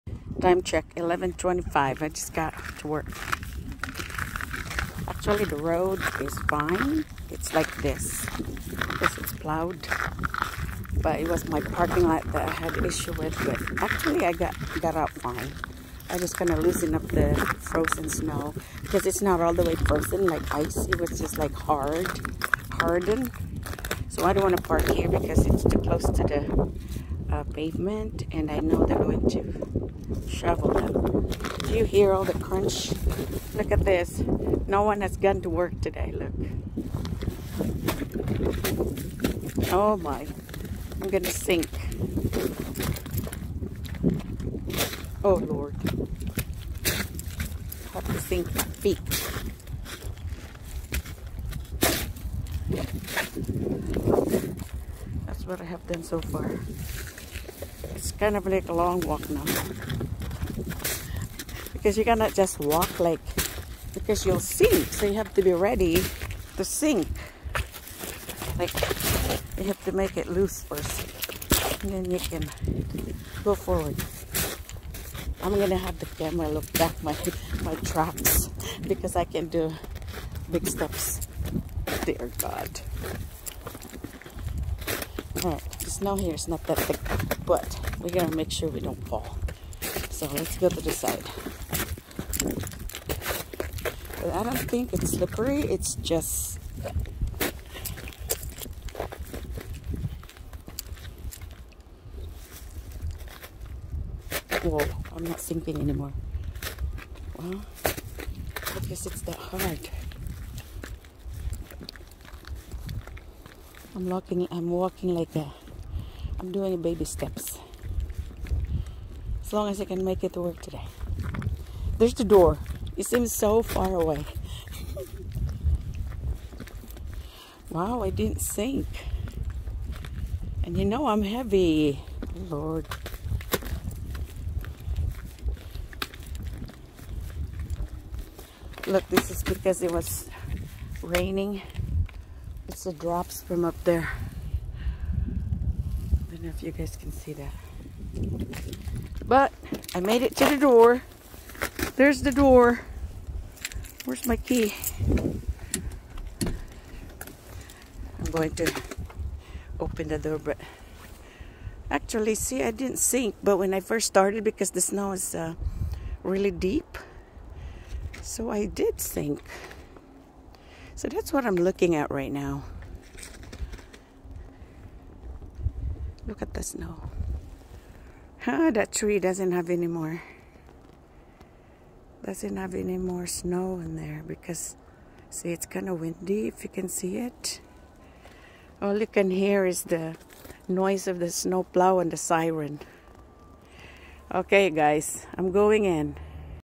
Snow + Freezing Rain
Walking on hardened snow due to freezing rain.